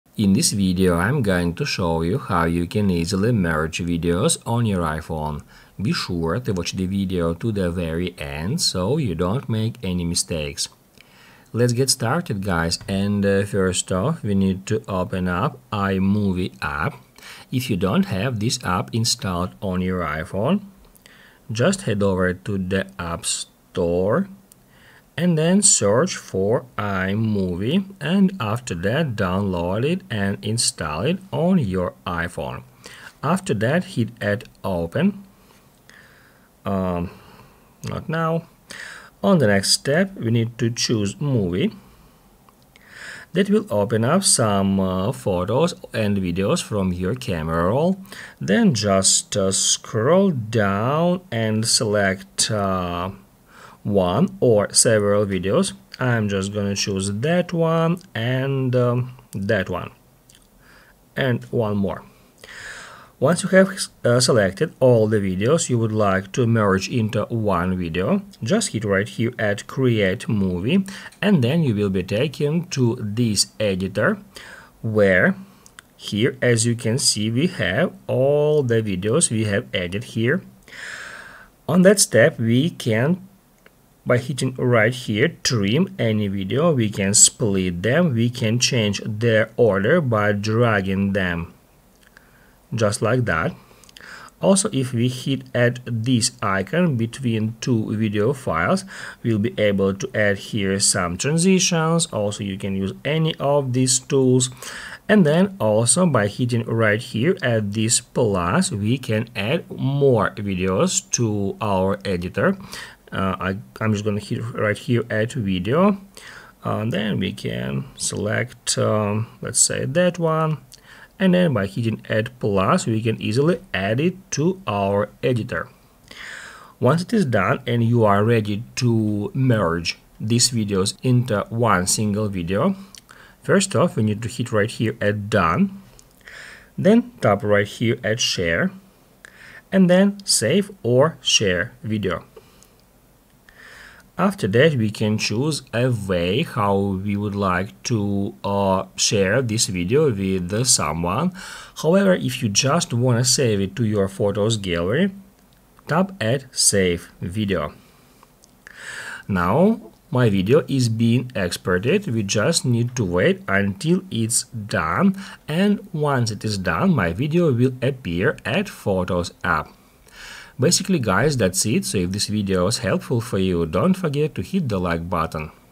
How to Merge Videos on iPhone
Here's how to merge videos on iphone! In this video I will show you how to combine videos on iphone. Be sure to watch the video to the very end. You will learn how to merge videos. That's easy and simple to do!